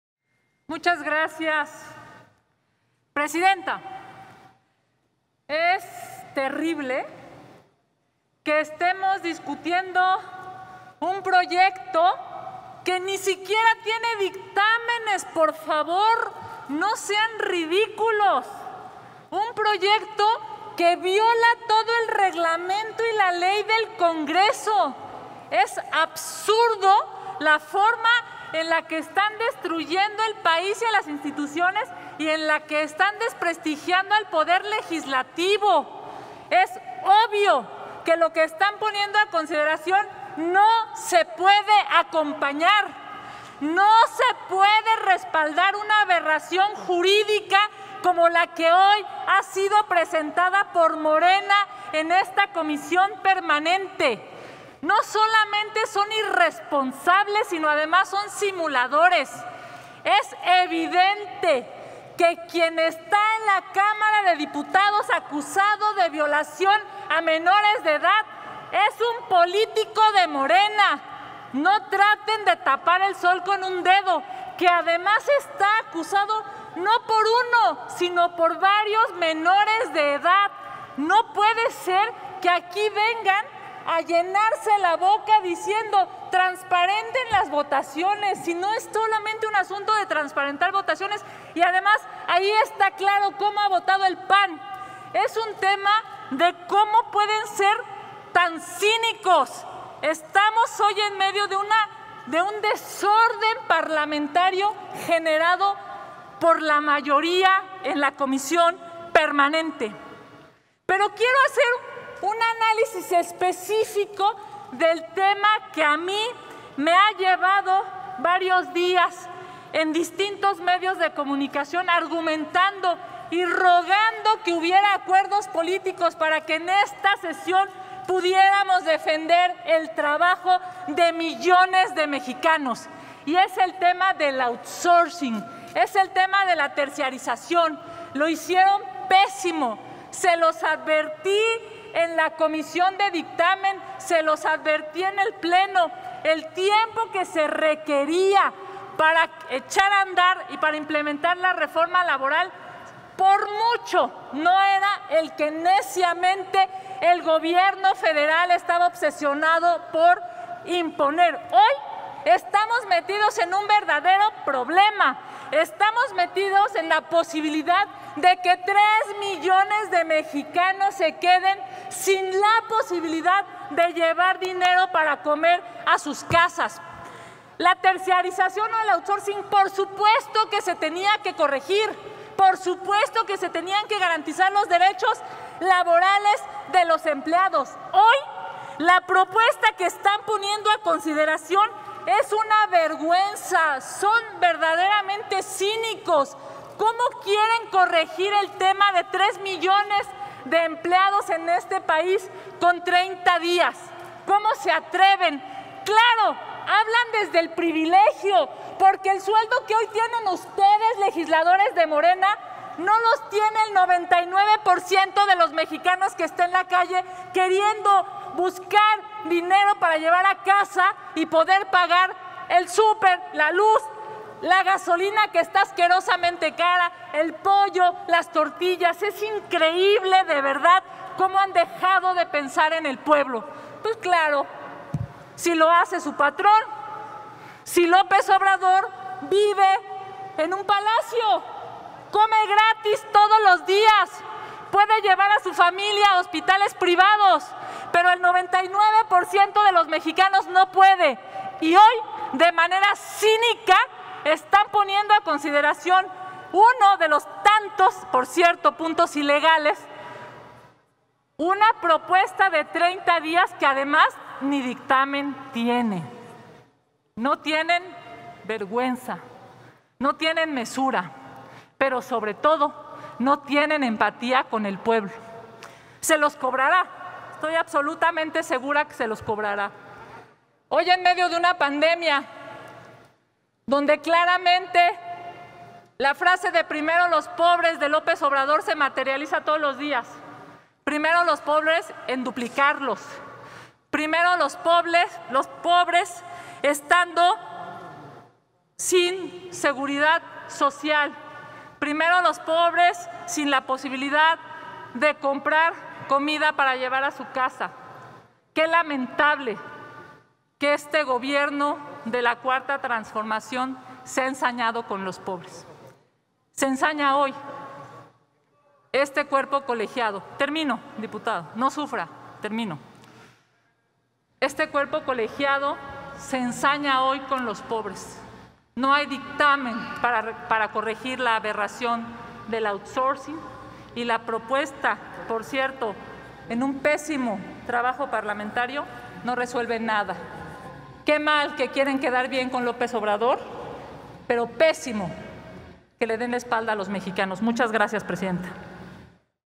Senadora Kenia López Rabadán, para referirse al dictamen en materia del periodo extraordinario
27 de julio de 2021.- Intervención de la senadora Kenia López Rabadán, al participar en la discusión de un dictamen de la Primera Comisión, por el que la Comisión Permanente del Congreso de la Unión convoca a la Cámara de Diputados a un primer periodo extraordinario de sesiones durante el segundo receso del tercer año de ejercicio de la Sexagésima Cuarta Legislatura.